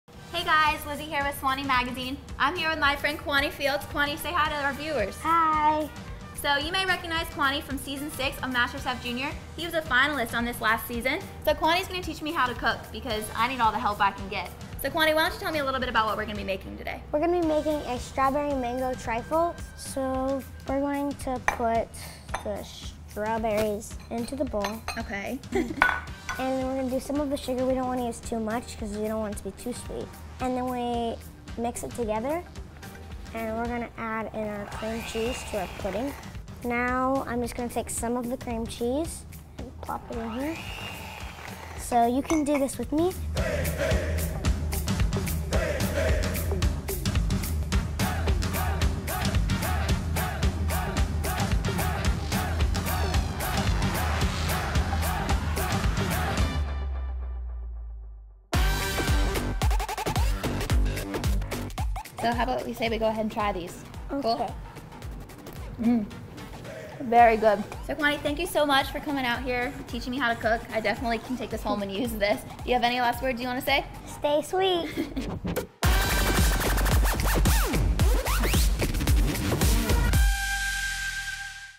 Suwanee Magazine gets a baking lesson from "Masterchef Junior" Finalist Quani Fields
We had the opportunity to meet Quani at our office,
where he taught us how to make a Strawberry
Mango Trifle Bowl.